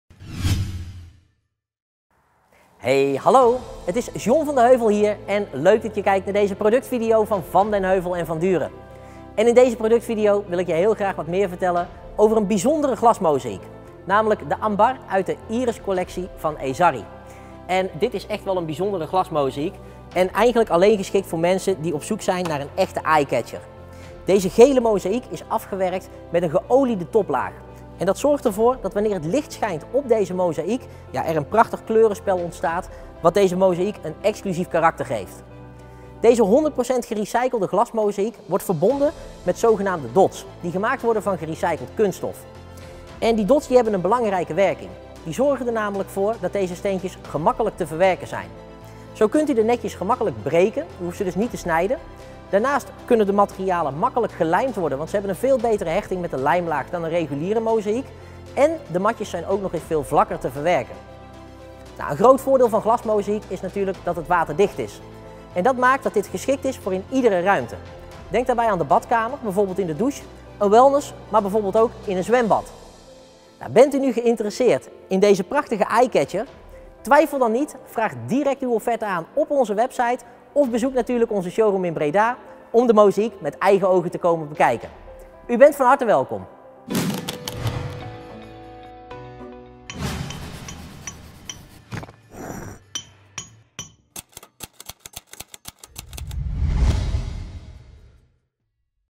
Iris Ambar | Geolied Glasmozaïek Glasmozaiek Ezarri | Badkamer wellness zwembad keuken geel gele
Voor meer informatie over dit product, klik op bovenstaande link.

Van Den Heuvel & Van Duuren – Indoor Glasmozaïek | Iris | Ambar.

De Ambar uit de Iris collectie van Ezarri met zijn lichte geel beige stukken Glasmozaïek welke perfect combineert met elk ander materiaal.

De gehele Iris collectie van Ezarri kenmerkt zich door zijn geoliede mozaïek steentjes. Door deze olie krijg je mooie kleurnuances, welke perfect toe te passen zijn voor:

- Een wellness, sauna of spa (Zowel particulier als zakelijk)
- Een Zwembad / Zwembaden (Zowel privé als voor een hotel)
- Badkamer of douche
- Toilet

Ook is dit glasmozaïek ideaal als decoratie elementen toe te passen voor uw keuken of woonkamer.
Doordat er licht op dit materiaal schijnt, krijg je diverse kleurnuances door de olie en past deze mozaïek perfect bij zowel landelijke als moderne ruimtes.

Onze serie glasmozaïeken worden vervaardigd uit duurzaam gerecycled glas, wat natuurlijk geheel milieuvriendelijk is.